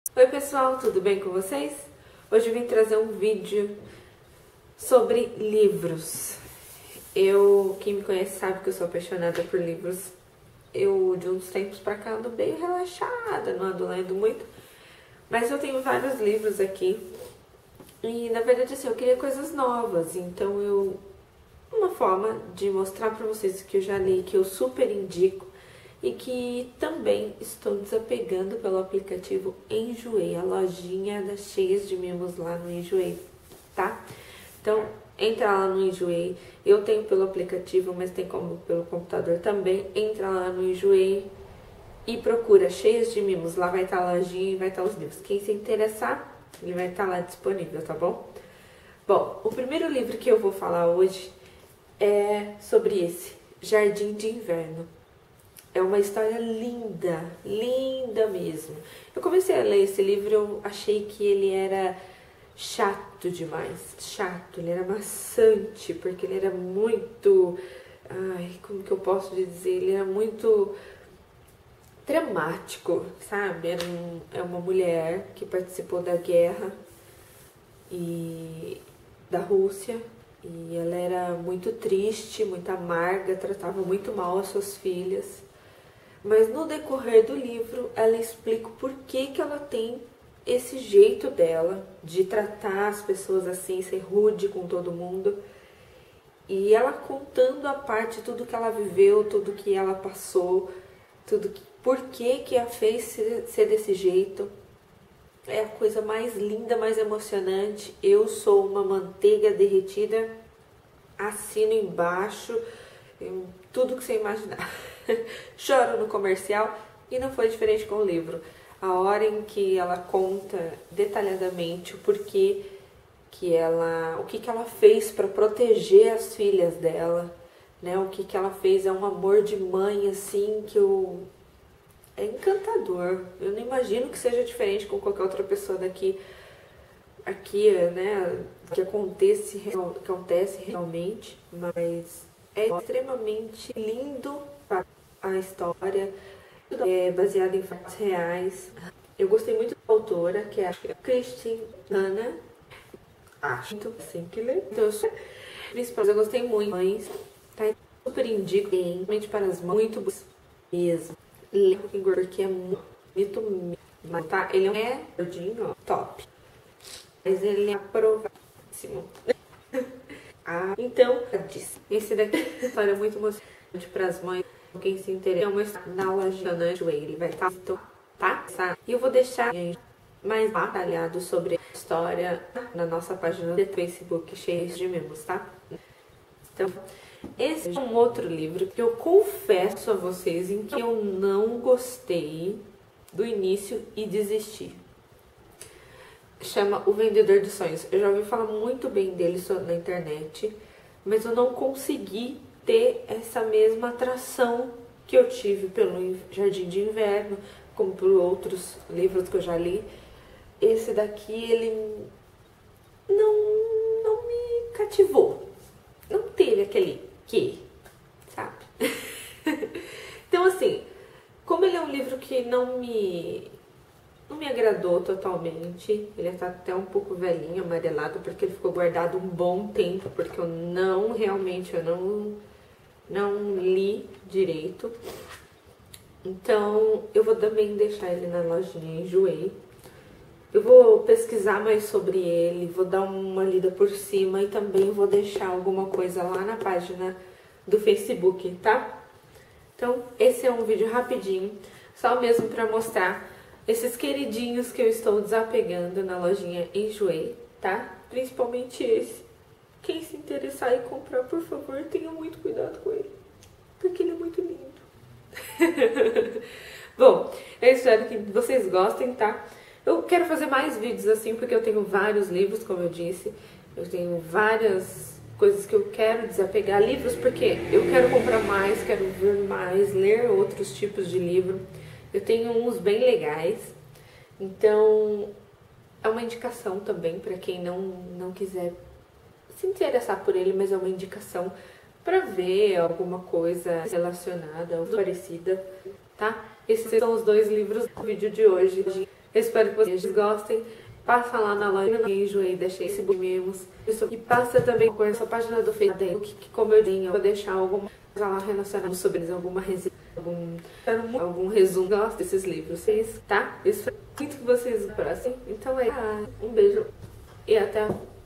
Indicações de Livros
Olá Pessoal, tudo bem? Gostaria de compartilhar com vocês os meus queridinhos de livros e já aproveitar e apresentar nossa lojinha no Enjoei.. Passa lá pra conferir, vou deixar na página do facebook uma resenha dos livros.. Os links estão aqui abaixo. Um beijo e até a próxima!!